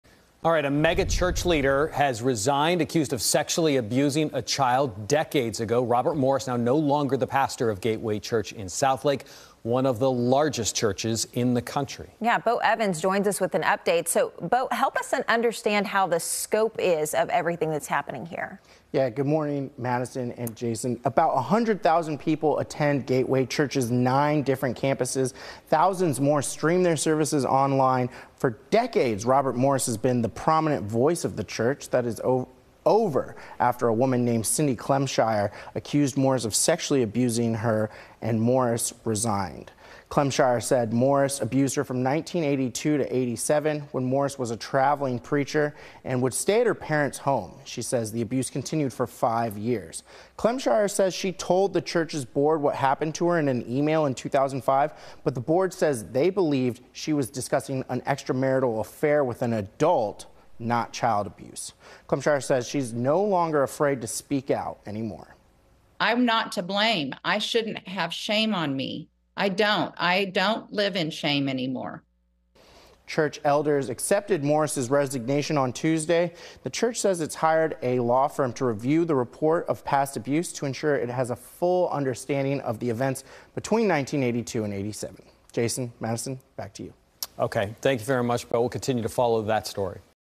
Gateway megachurch pastor Robert Morris resigns
Robert Morris is no longer the pastor of the Southlake church after a woman accused him of sexually abusing her as a child in the 1980s.